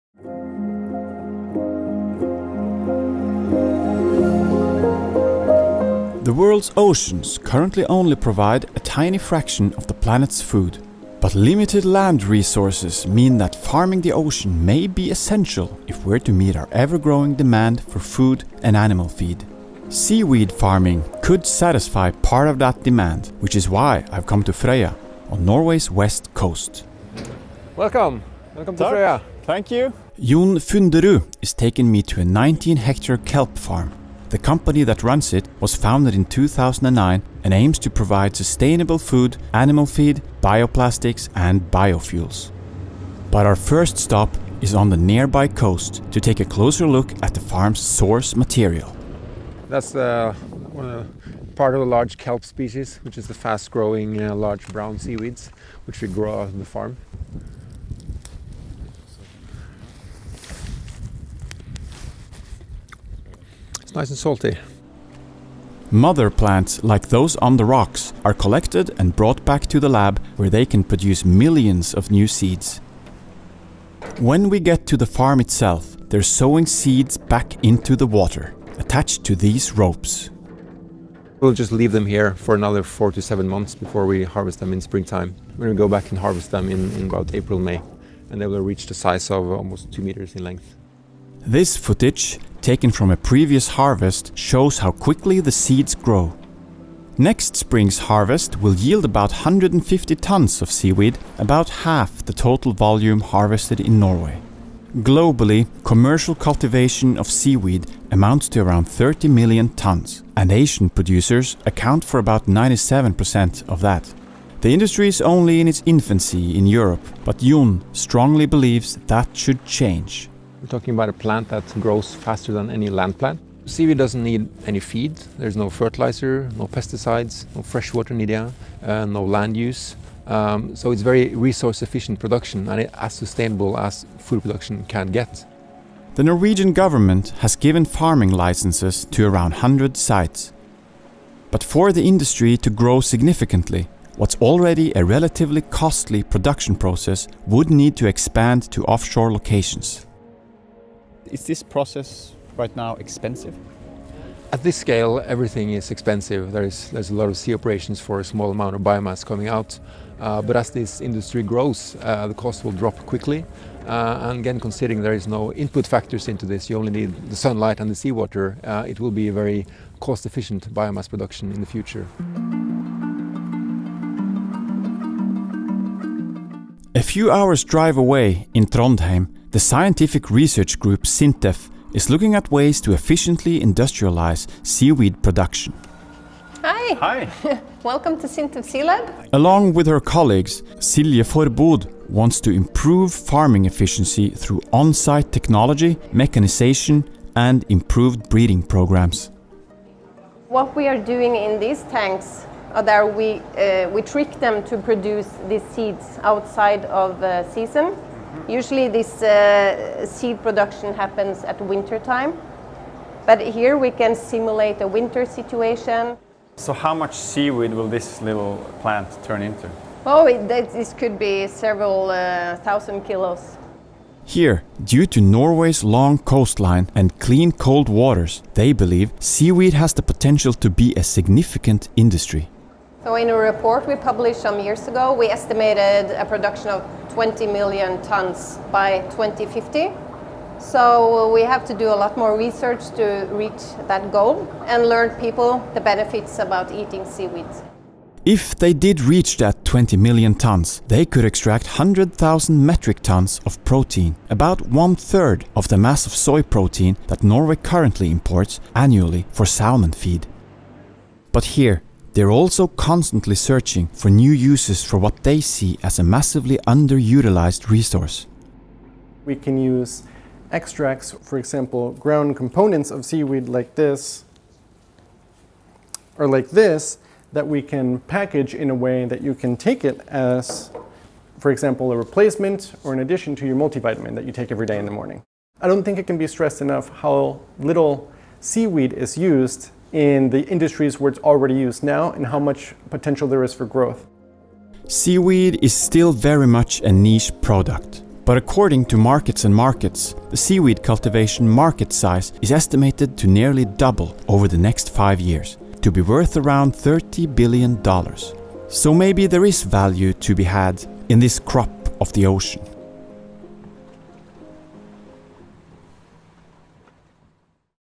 O cultivo de algas é uma opção de alimento sustentável para o futuro?
Os recursos limitados de terra sugerem que o cultivo de algas marinhas pode ter papel relevante no atendimento à crescente demanda por alimentos.